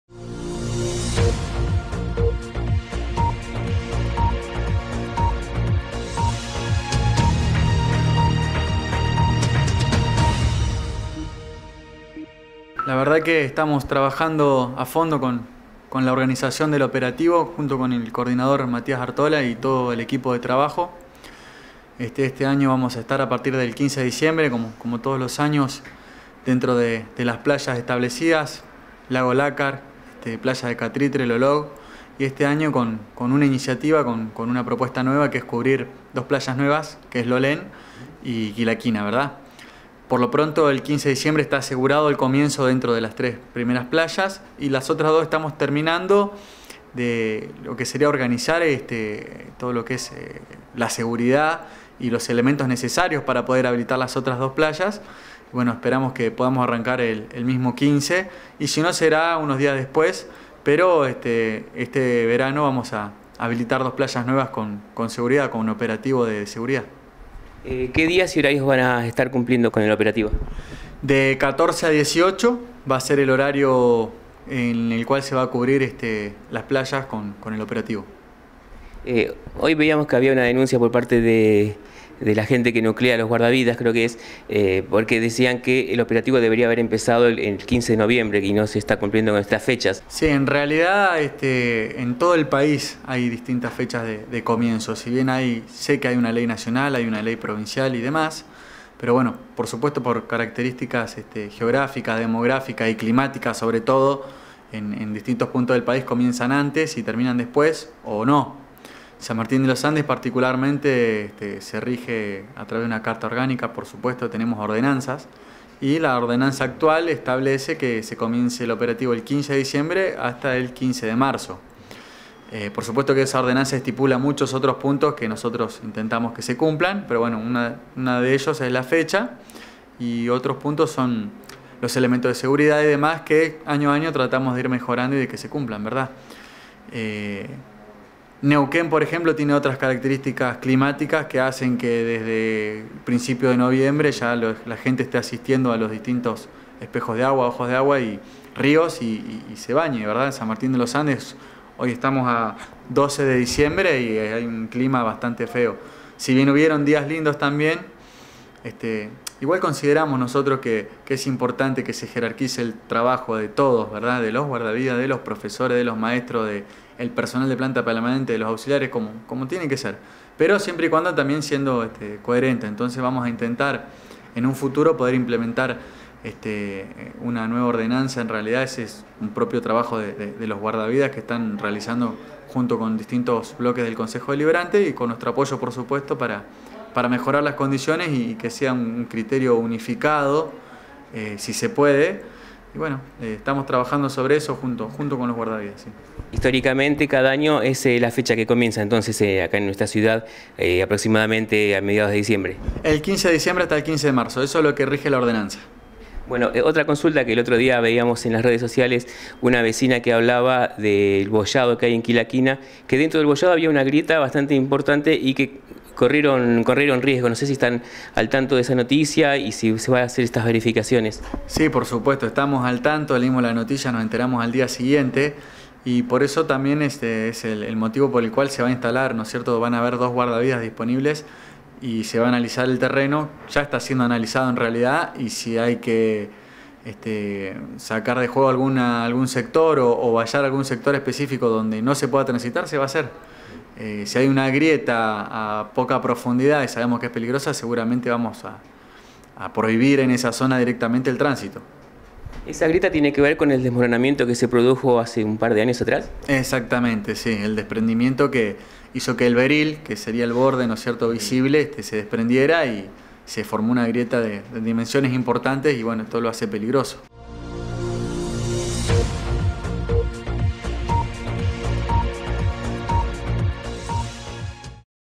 El Operativo de Guardavidas comenzará el 15 de diciembre hasta el 15 de marzo
SMAP - 2016
El Subsecretario de Deporte Gerardo Sanabria, confirmó que el jueves 15 de diciembre, se pondrá en marcha el Operativo de Guardavidas en las playas del Lago Lacar, Costanera y Catritre y Playa Bonita en Lolog, además adelantó que éste año, se sumará el servicio de guardavidas en Lolen y Quila Quina, aunque podría demorarse algunos días más, por cuestiones de organización.
Sobre la denuncia de SIGUNE, (Sindicato de Guardavidas de Neuquén) por incumplimiento en la fecha de inicio de operativo, considerando que debe iniciarse el 15 de noviembre, Sanabria aclaró que si bien la Ley Nacional invita a los Municipios a comenzar en ese fecha, cada lugar se adecua a las características geográficas, demográficas y climáticas. Históricamente y de acuerdo a la Ordenanza Municipal, el Operativo de Guardavidas se realiza desde el 15 de diciembre hasta el 15 de marzo.
También se refirió a una situación vivida por dos vecinas en una zona boyada de Quila Quina, donde se encontraron con una grieta que puso en riesgo sus vidas, “si hay una grieta a poca profundidad y sabemos que es peligrosa, seguramente vamos a prohibir en esa zona directamente el tránsito”, señaló el Subsecretario de Deporte.-